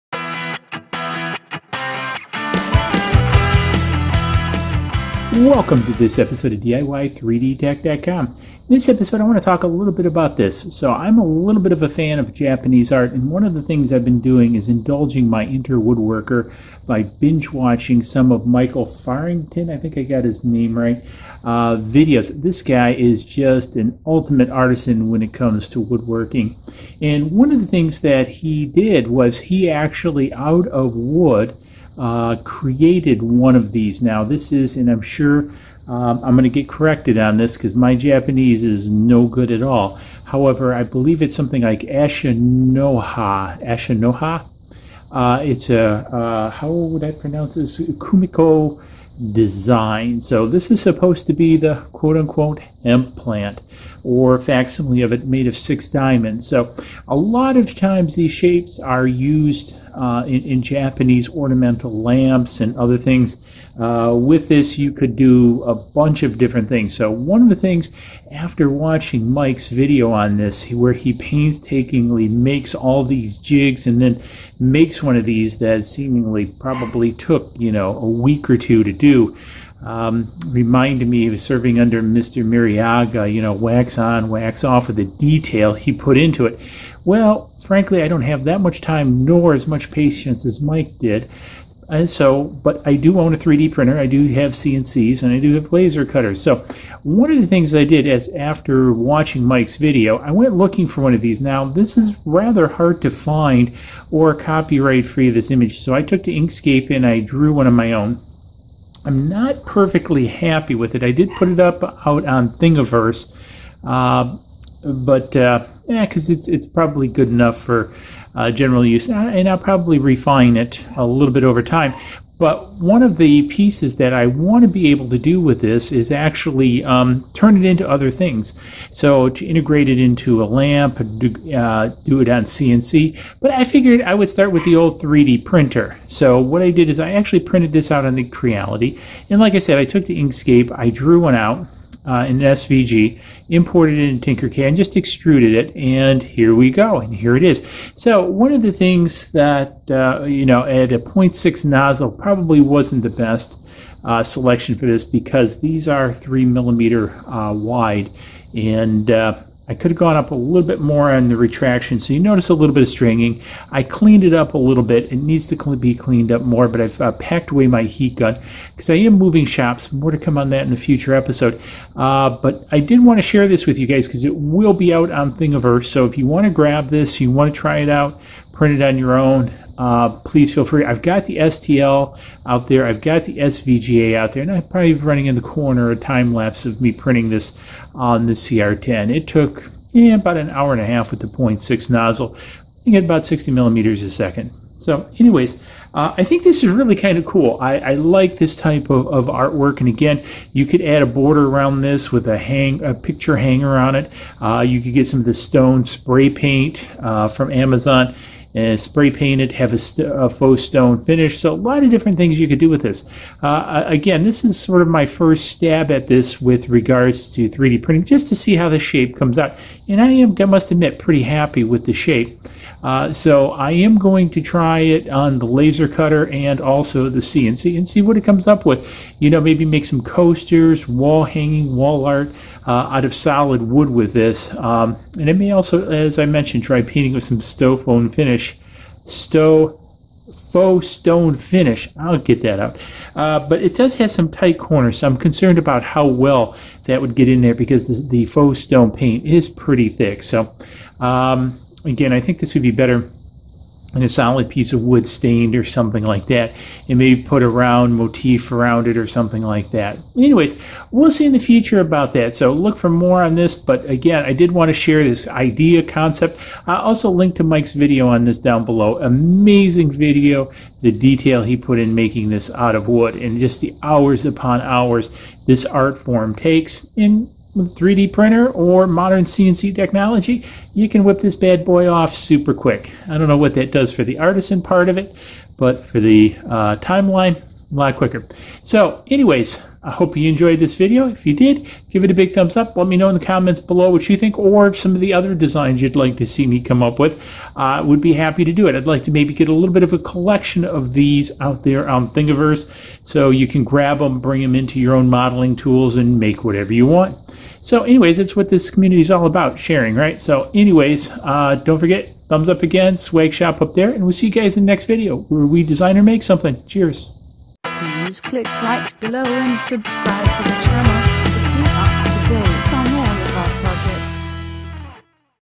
Design Project 3D Printing Kumiko AsaNoHa Pattern!
In this episode, we will be 3D printing an Asanha, Kumiko pattern we designed in Inkscape and imported into Tinkercad to create an STL!  This started with me watching Mike Farrington's video (see more ) and he just blows me away with the dial in his work.  Now I have neither the time nor patiance Mie does but I do know Inkscape and have a 3D printer, Laser Cutter and CNC!   So I drew up a rough image as that was even harder then looks and created SVG file.  I then 3D printed this for starters and placed it out on Thinigiverse (see more ) in case someone else want to use this design for a project!